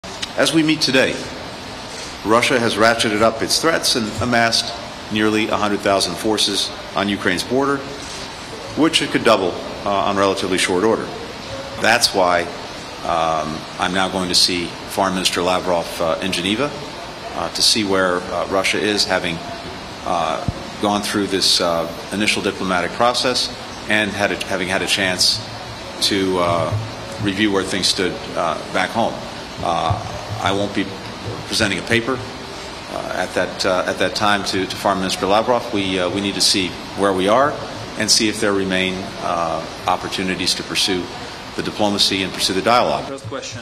Russia Has 100,000 Troops on Ukraine Border: Blinken
Secretary of State Antony Blinken says Russia has amassed 100,000 troops on the border with Ukraine. He spoke in Ukraine before heading to meet his Russian counterpart in Geneva.